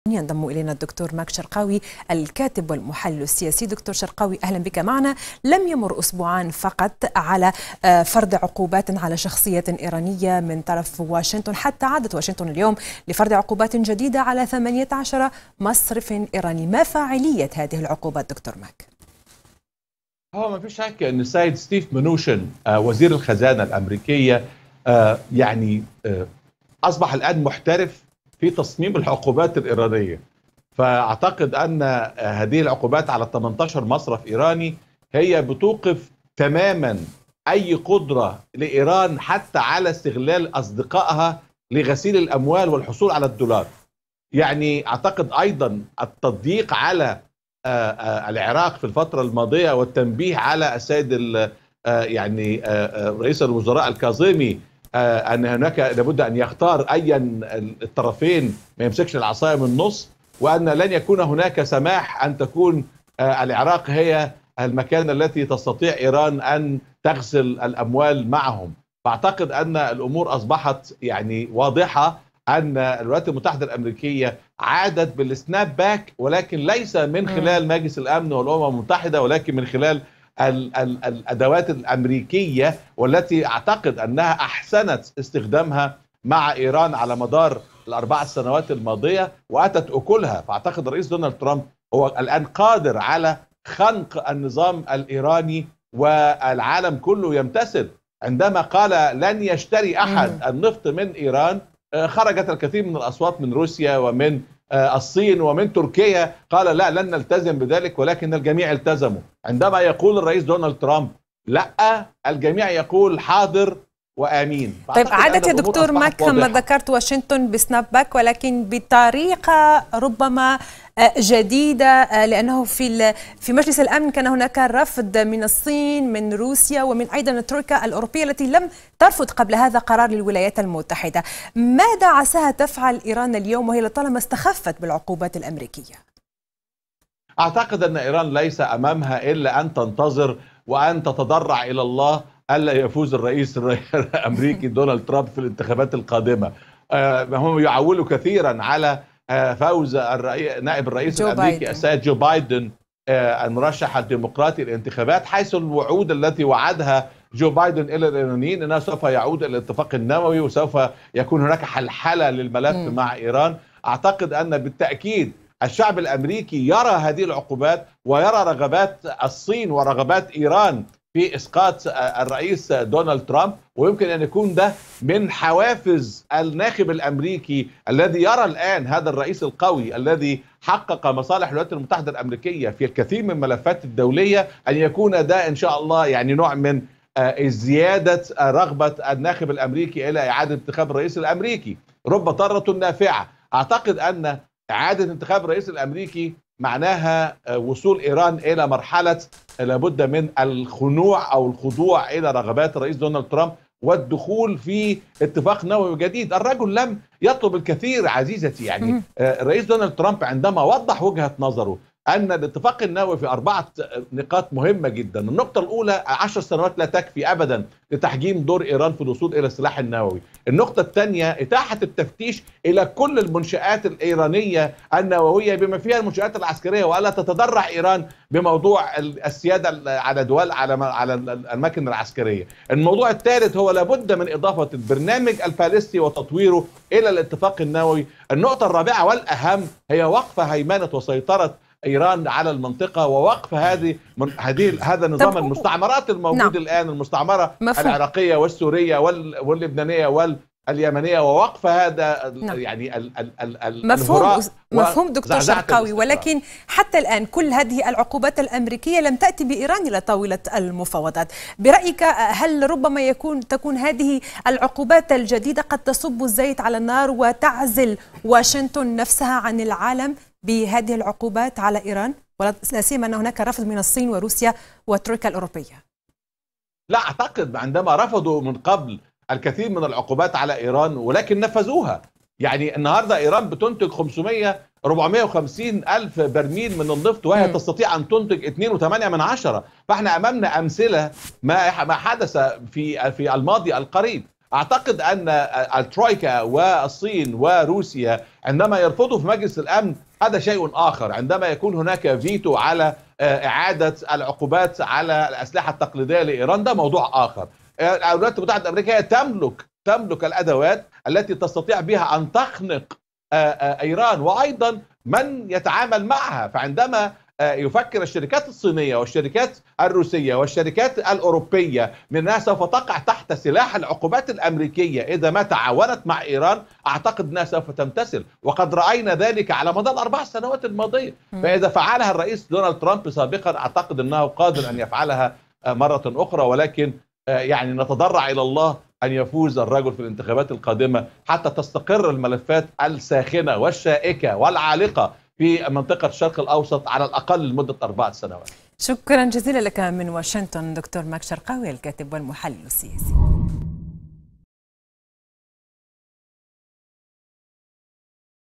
بعد العقوبات على 18 مصرفا.. محلل: ترامب الآن قادر على خنق النظام الإيراني
أعلنت الولايات المتحدة فرض عقوبات على ثمانية عشر مصرفا إيرانيا رئيسيا. وقال وزير الخزانة الأمريكي ستيفن منوتشين إن العقوبات ستستمر إلى أن تتوقف إيران عن دعم الأنشطة الإرهابية وتضع حداً لبرامجها النووية.
المزيد من التفاصيل مع دكتور ماك شرقاوي  الكاتب والمحلل السياسي عبر النشرة الإخبارية مع الإعلامية منى بلهيم على شاشة قناة الغد.